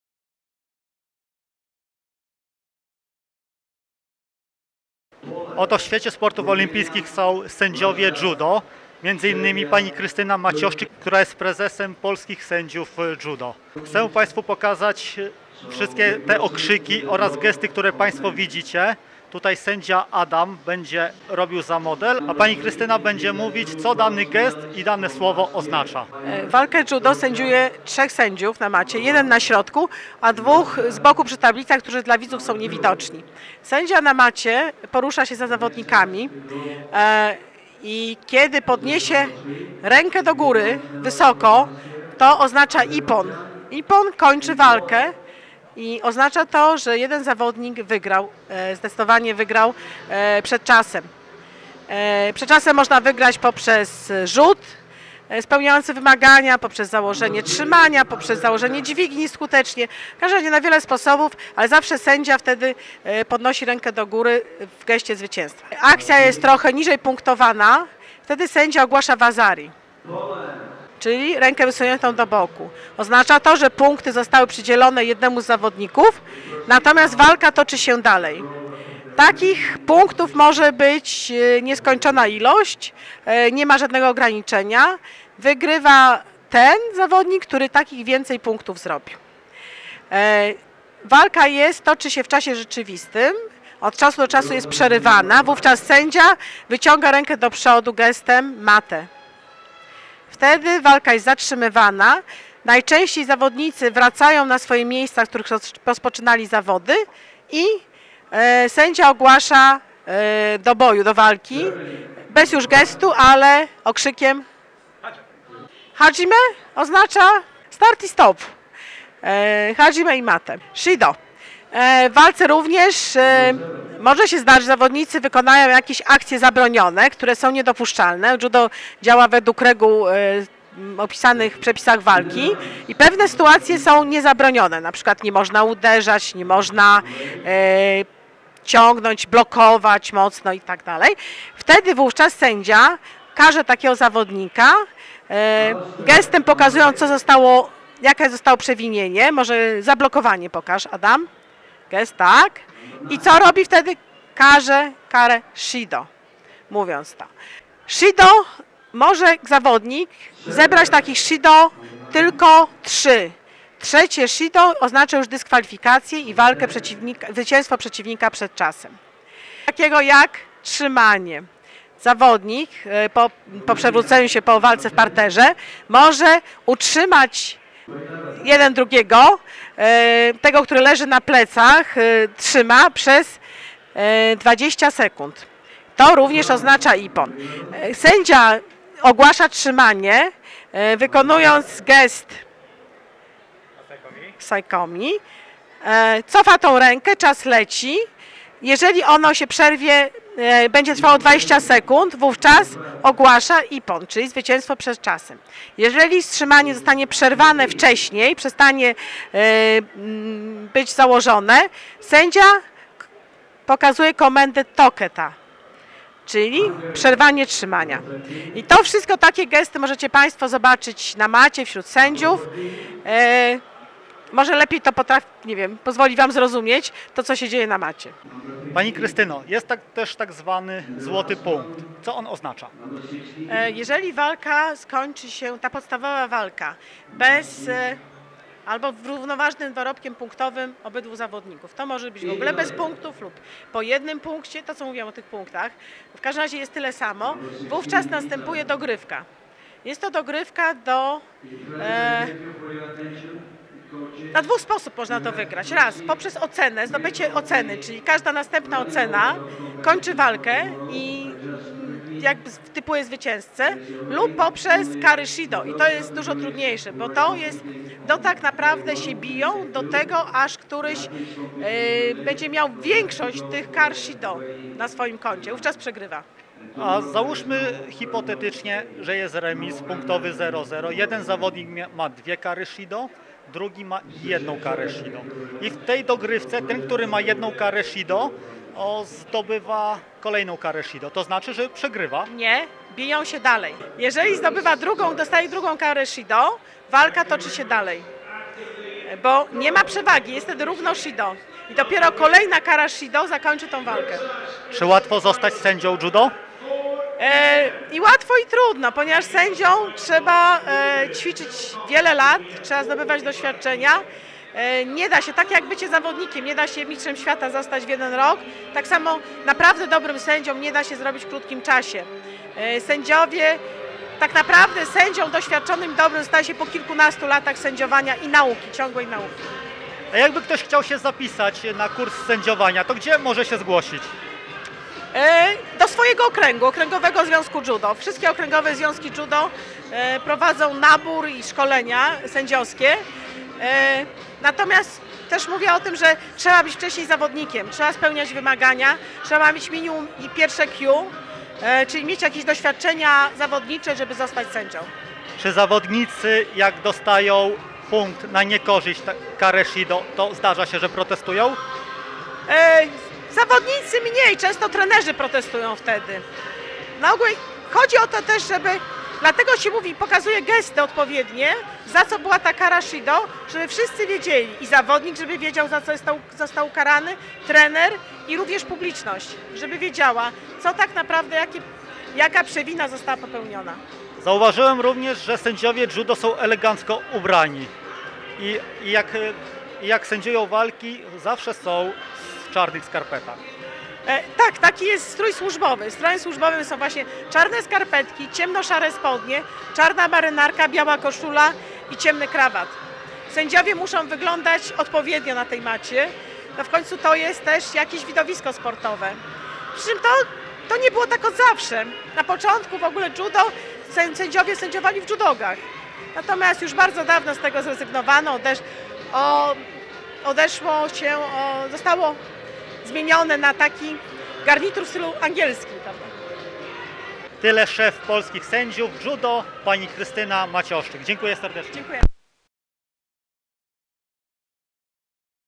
Sędziowanie judo
Przybliżenie pracy sędziowskiej w judo